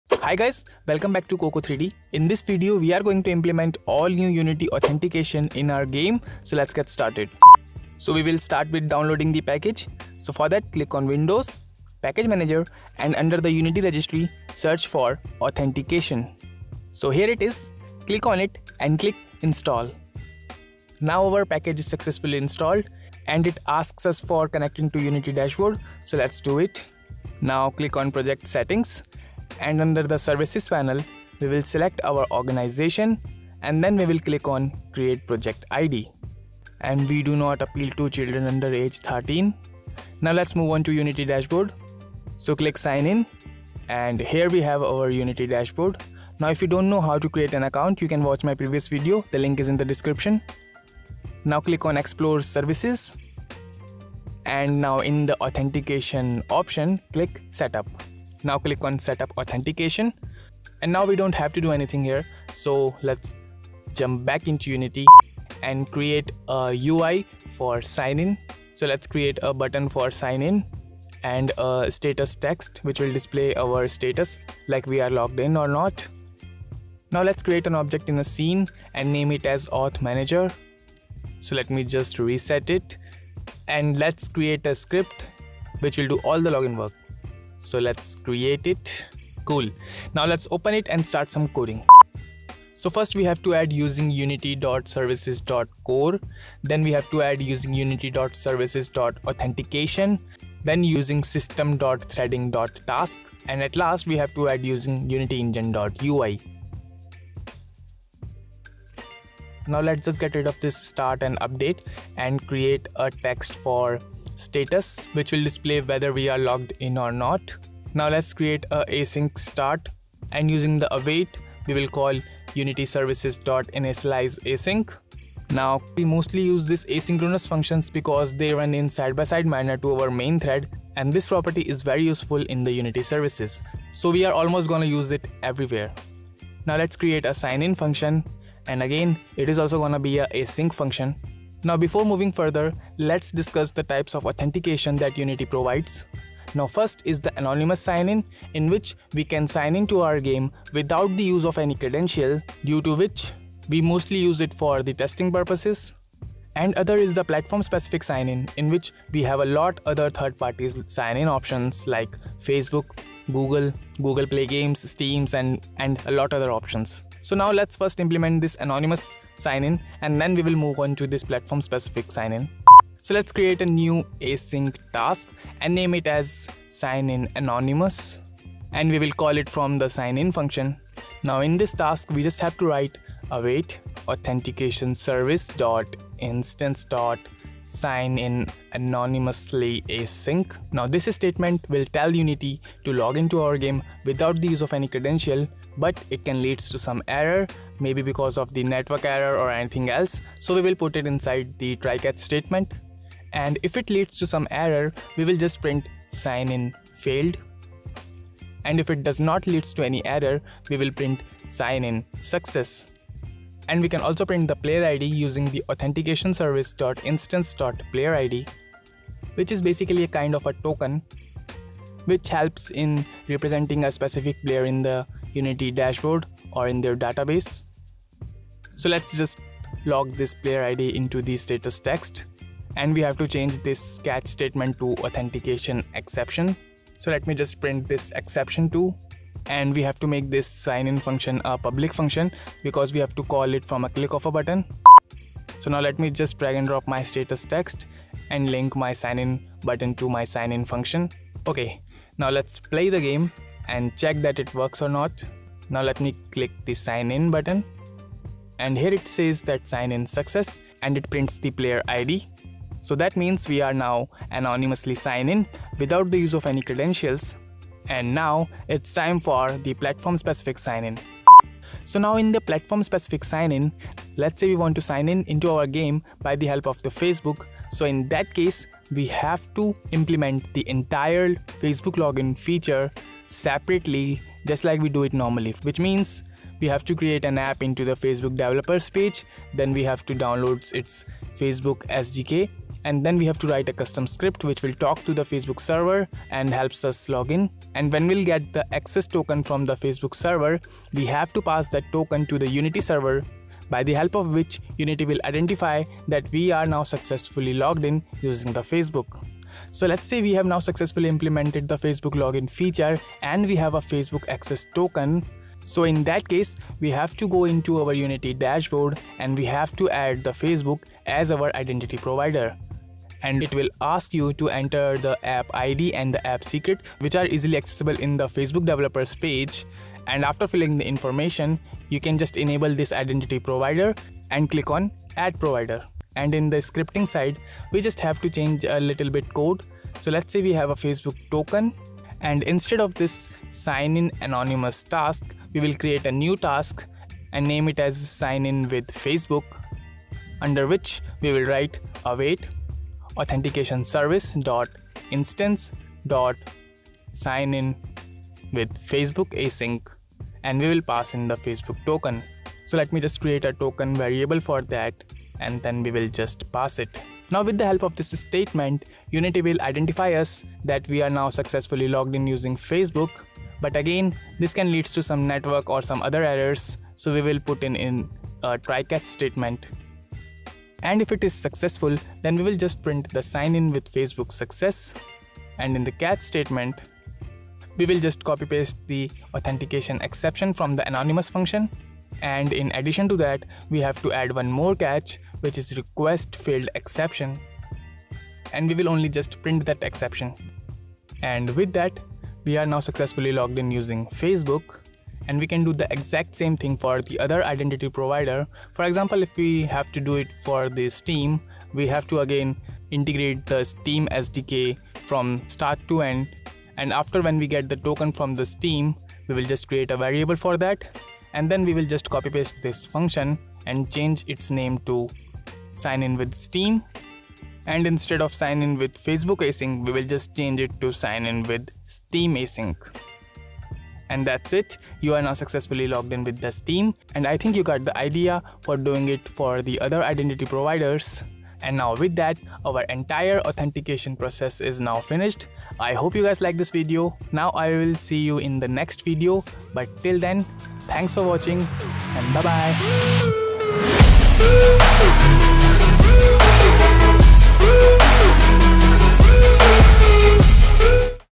AUTHENTICATION IN UNITY Easy Integration for BEGINNER! 2023 UNITY GAMING SERVICES
🎥💥Hi Guys,  This is  the ultimate guide to implementing the all-new Unity Authentication, from start to finish.

In this Short and simple tutorial, we'll walk you through every step of the Unity Authentication implementation process. Whether you're a seasoned developer or a curious beginner, This video has got you covered!